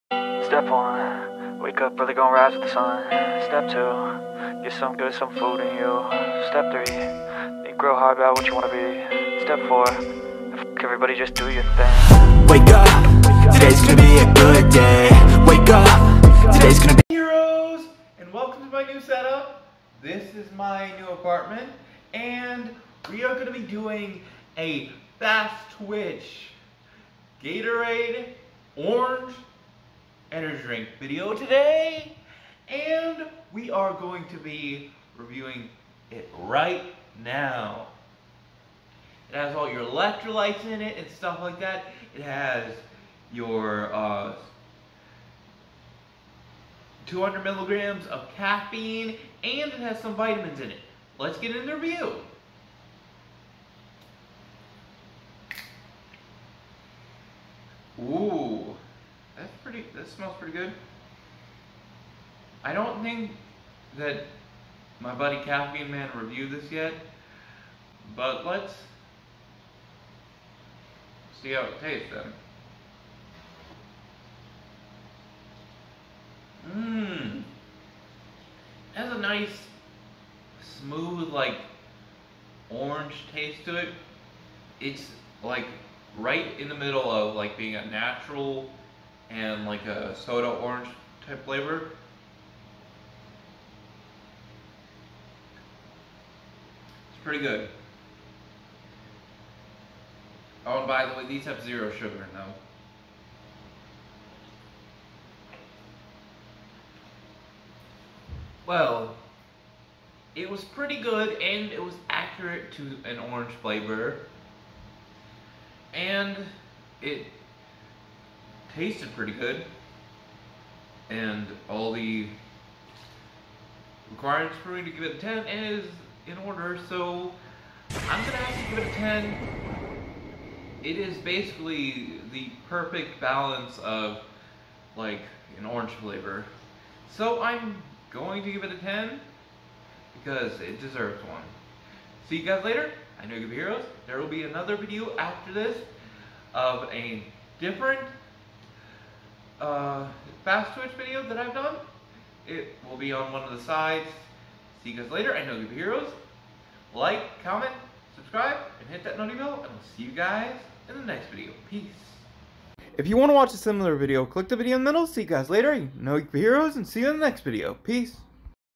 Gatorade Fast Twitch Orange Energy Drink Review
See I Told You I'd Find Another To Review But I Didn't Think It'd Be This Soon

You Can Find Pretty Much All Gatorade Fast Twitch Items On Campus Protein